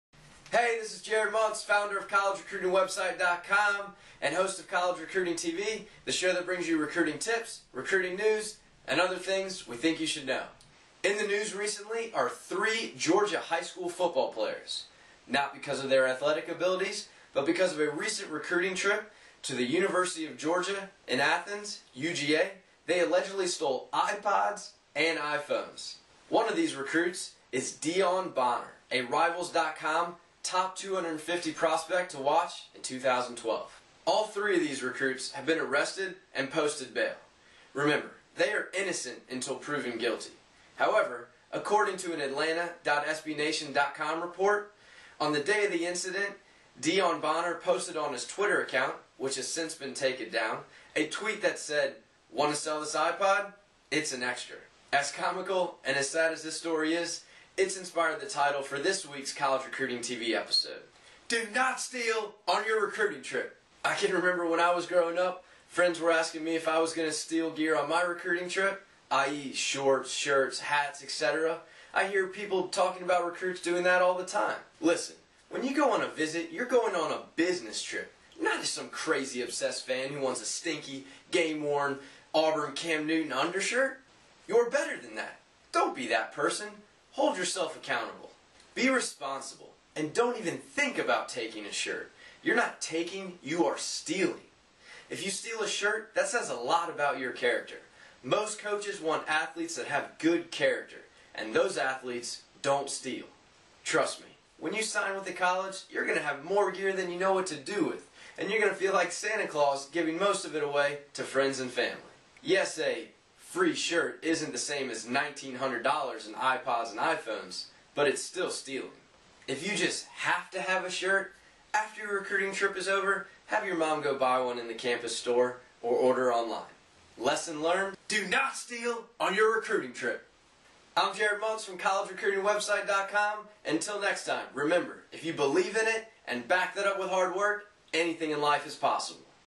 Do NOT STEAL on your Recruiting Trip! - College Recruiting TV
Do NOT STEAL on your Recruiting Trip is the title of this week's episode of College Recruiting TV.  This episode was inspired by the recent allegations of three Georgia High School Football recruits that stole ipods and iphones on their recent recruiting trip to UGA - University of Georgia in Athens.

Find more recruiting news, tips and download our free College College Recruiting Handbook

Say Hello!